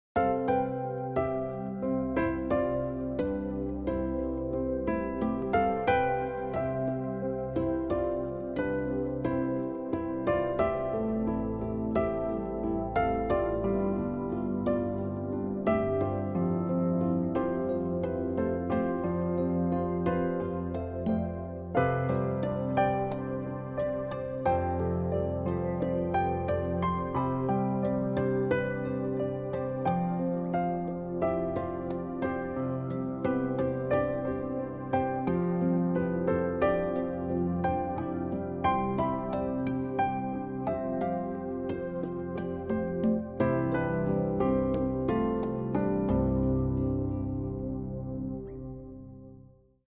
stary night time lapse Jan 13 14
2500 picture make up this very cool time lapse of night sky